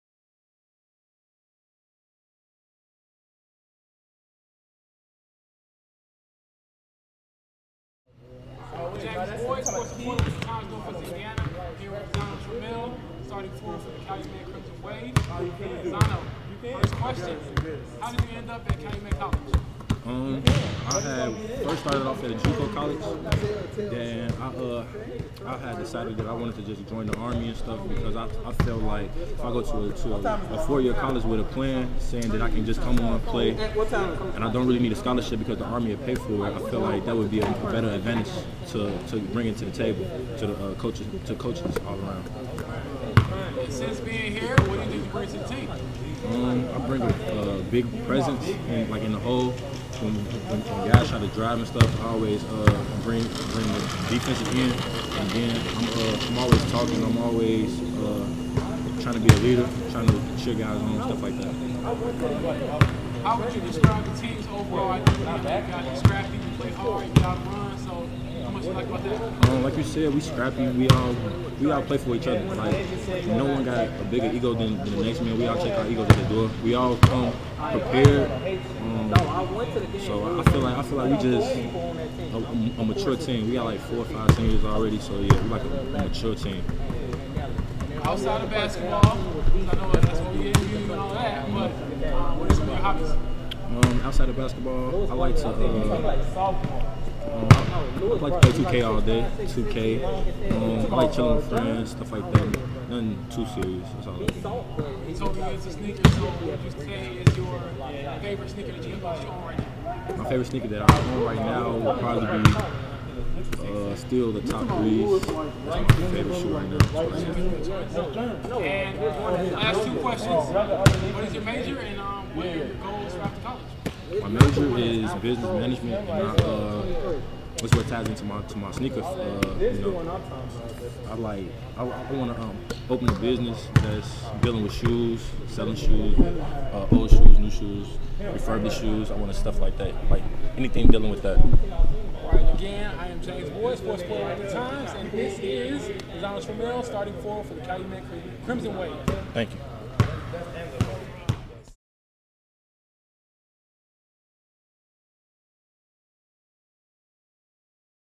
Lazono Trammell, Calumet College of St. Joseph Crimson Wave Forward
Lazono Trammell is a starting forward for the Calumet College of St. Joseph Crimson Wave men's basketball team.

Interview by James Boyd. Video produced by Kale Wilk.